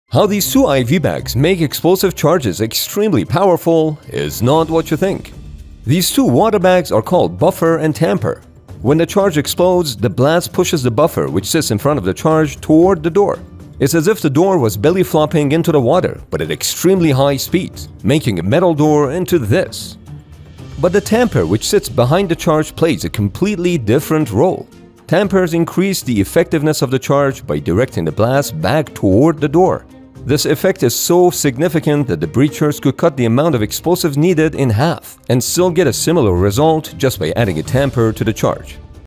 Improving Explosions with WATER
Yes you can use regular IV bags with water to make explosion so much larger